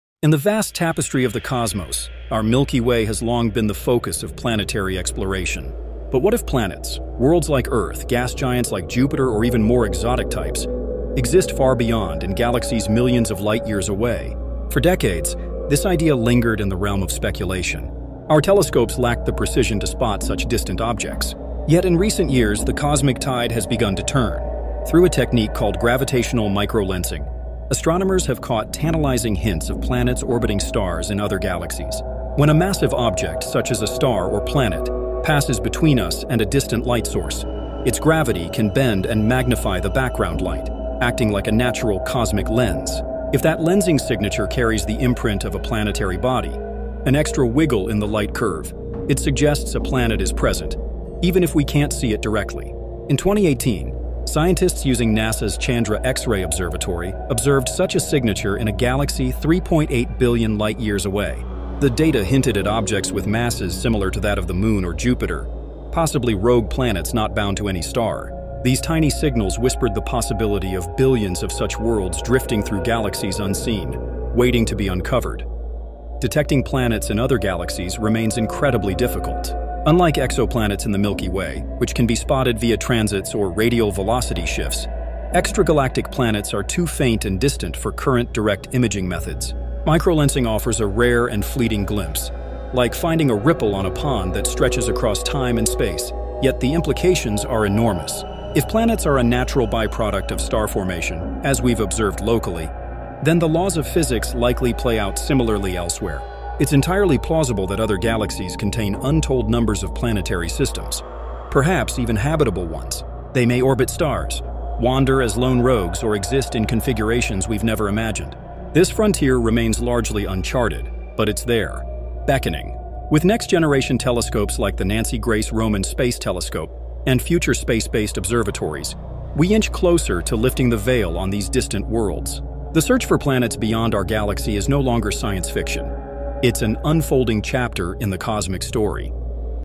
Worlds Beyond Our Reach: The Hunt for Planets in Other Galaxies
Are there planets beyond our galaxy? This video explores the groundbreaking science of microlensing and the thrilling search for extragalactic worlds. Discover how astronomers are uncovering distant, unseen planets across the universe.